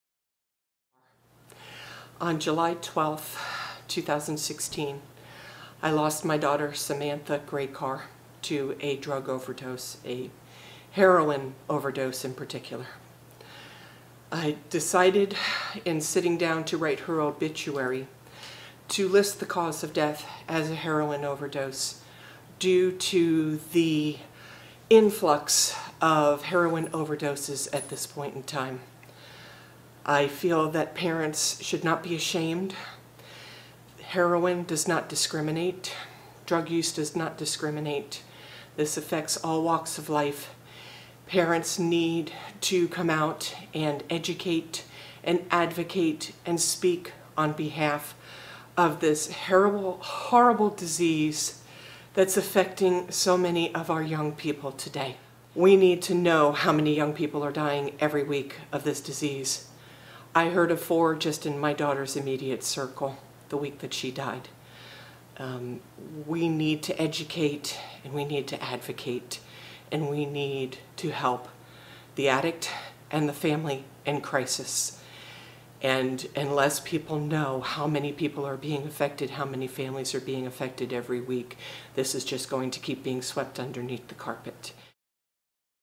VIDEO: Kim Grajcar's Daughter Died of a Heroin Overdose
Kim Grajcar’s daughter Samantha died of a heroin overdose. She discusses why she feels it is important for families to be open about cause of death in obituaries. (Bruce R. Bennett / The Palm Beach Post)